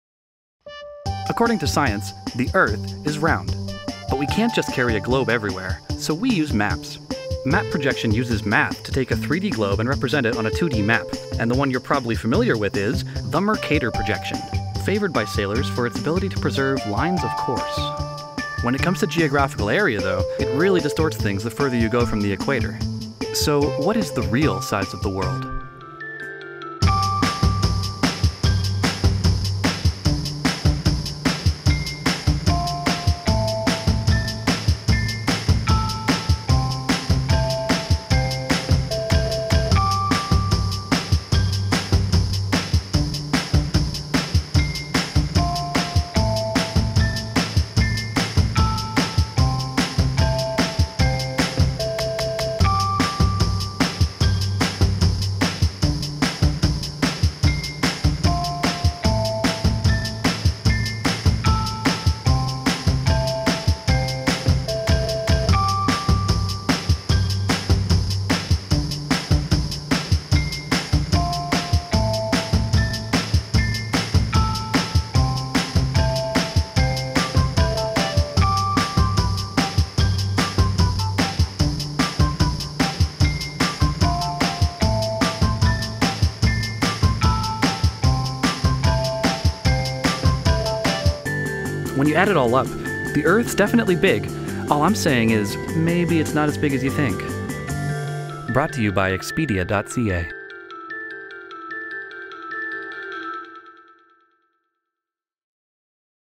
The Real Size of the World
Is the Mercator Projection map we're used to lying to us? Although it's the most commonly used map today, when it comes to geographical area it can be very misleading. Let's find out how big the world really is.